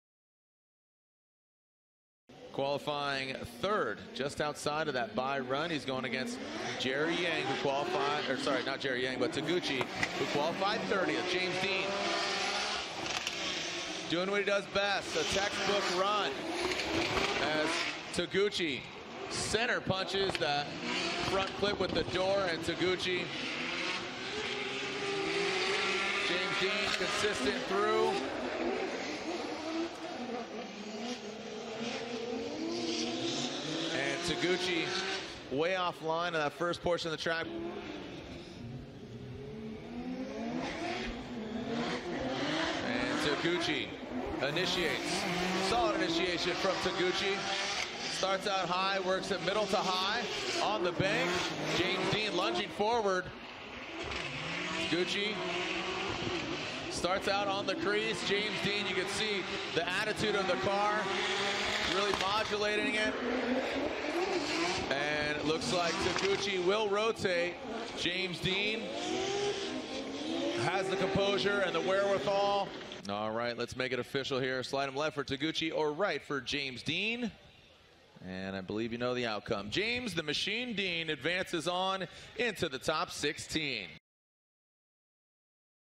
James Deane Vs Kazuya Taguchi (Wall - 2018)
Formula Drift Round 4 The Gauntlet Wall, NJ